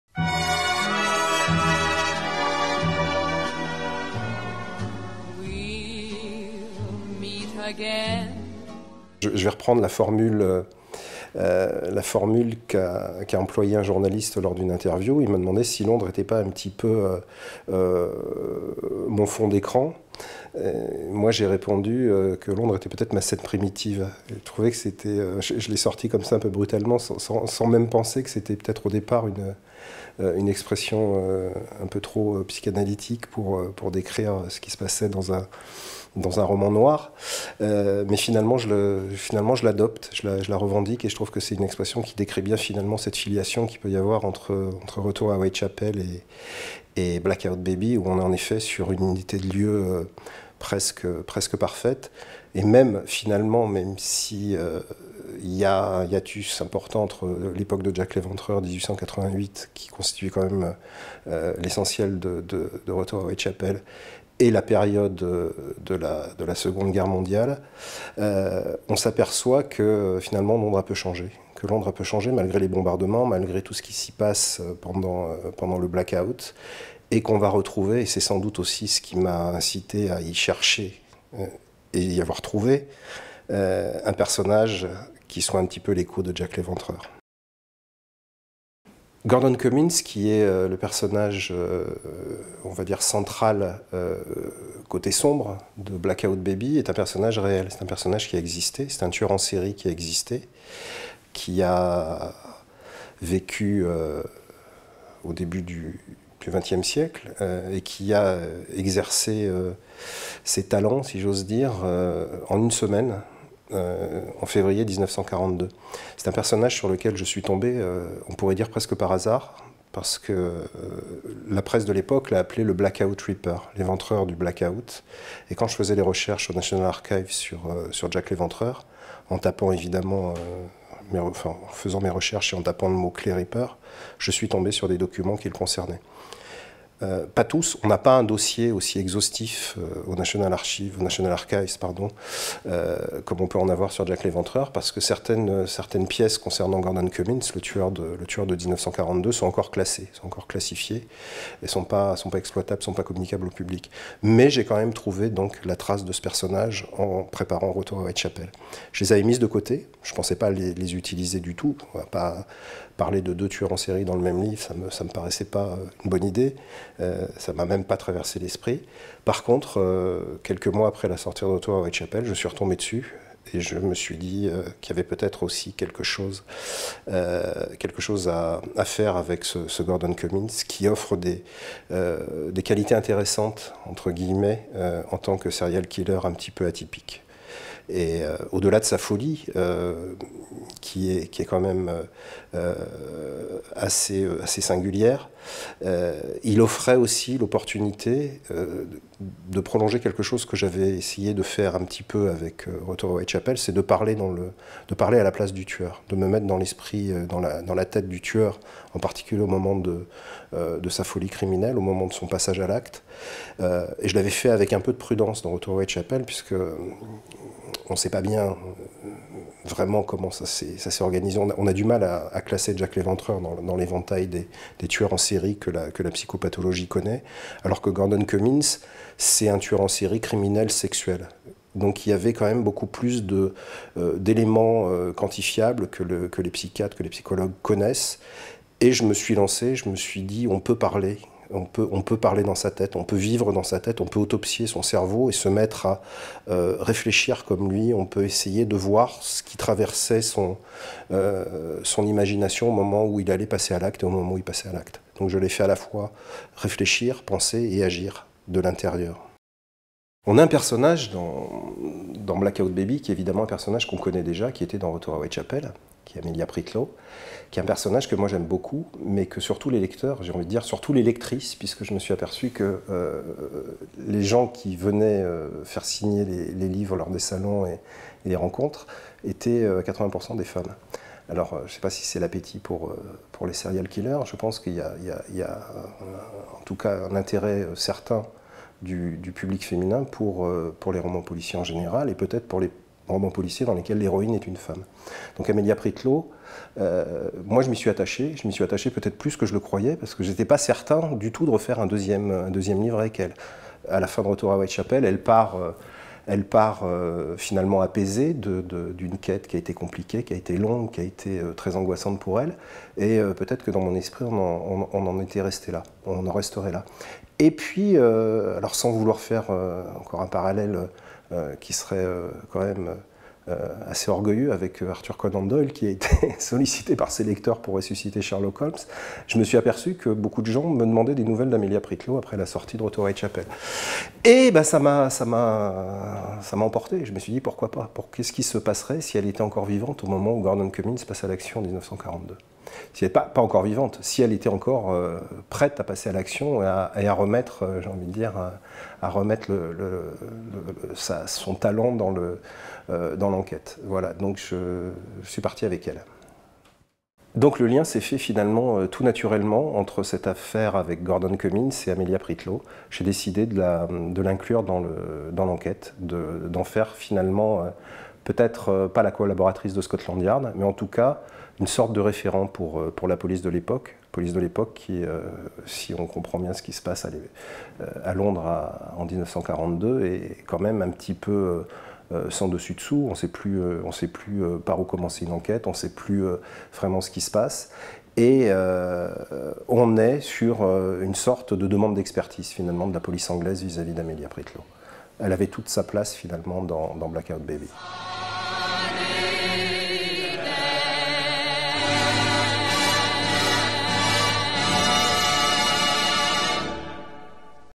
Michel Moatti - Blackout baby
Michel Moatti vous présente son ouvrage "Blackout baby, Londres, 1942" aux éditions HC.